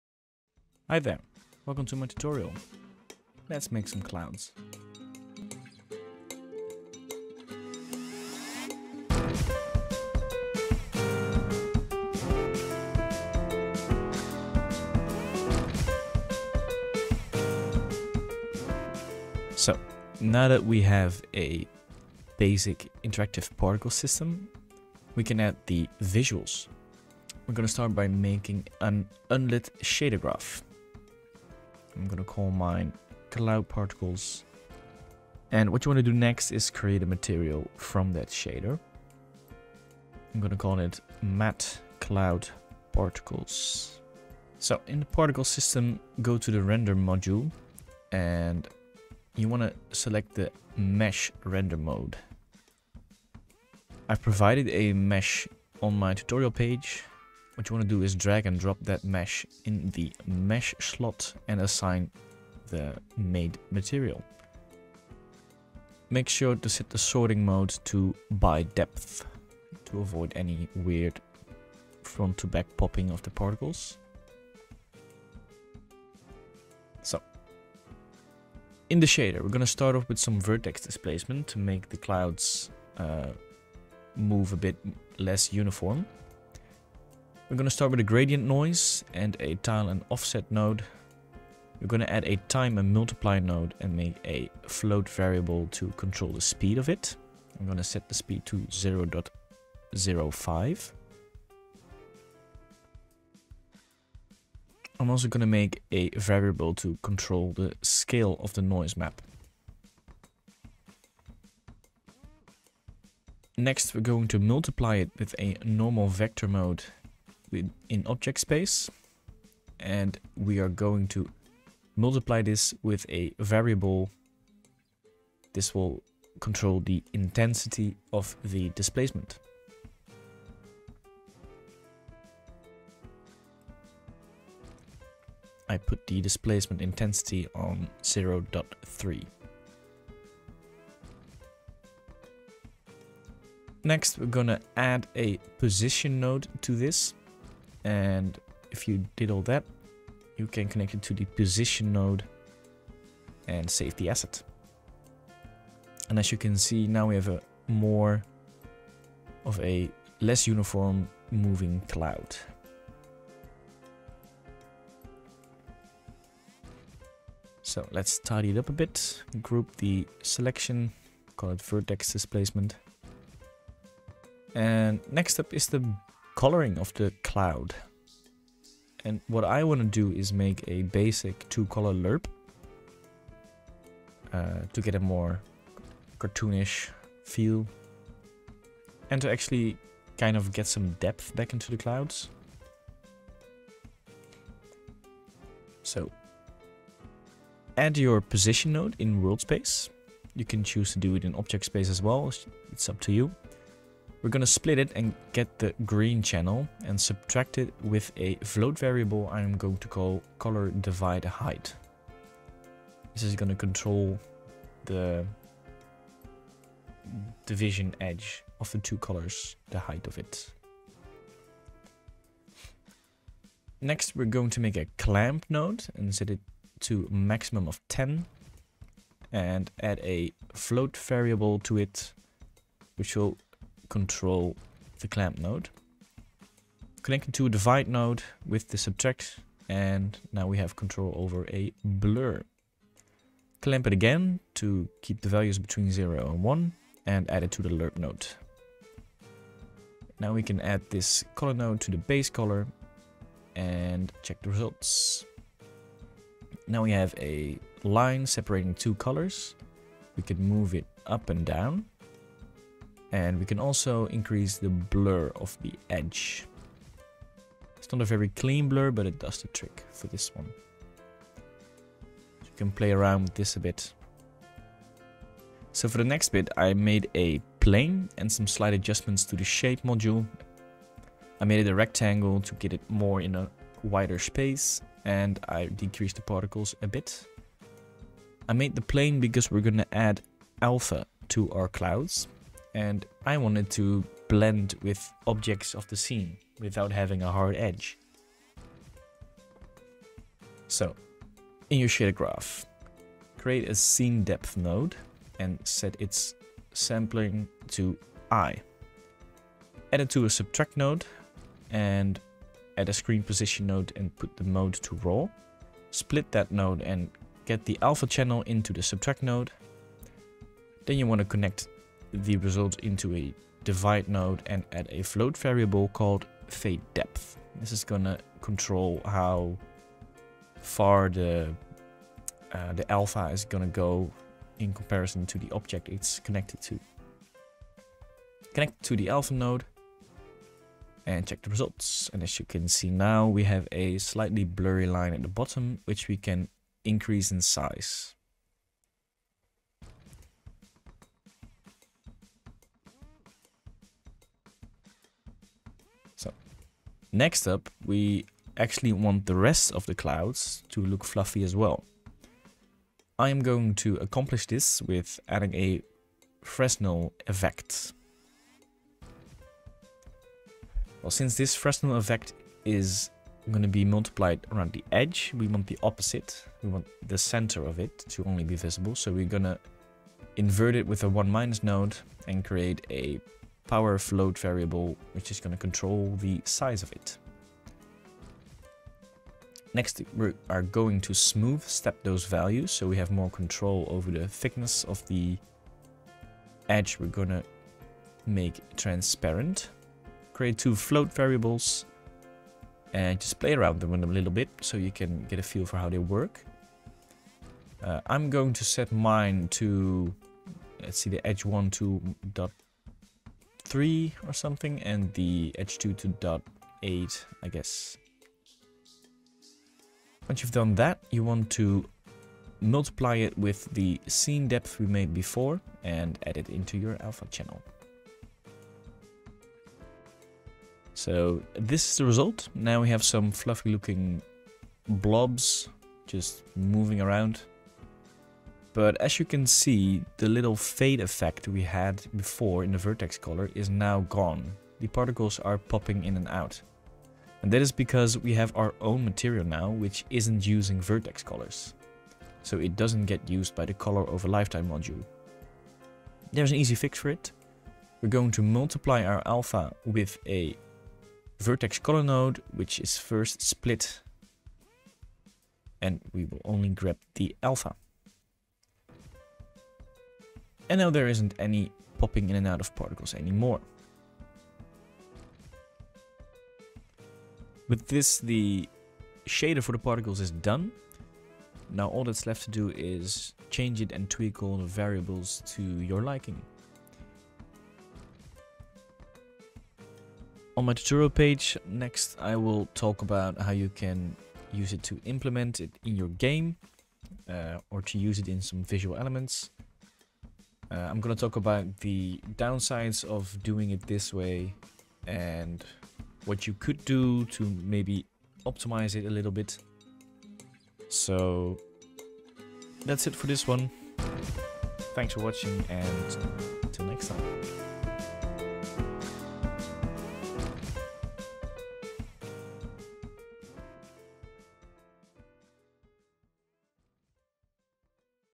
Unity Interactive Cloud Particles | Building The Shader (part 3)
Hi! Thanks for checking out my tutorial.

Tutorial Site:
Since the google site has dissapeared for some reason I can only offer the assets used from my drive:

This tutorial was made for an interactive particle system for the game All Hands on Deck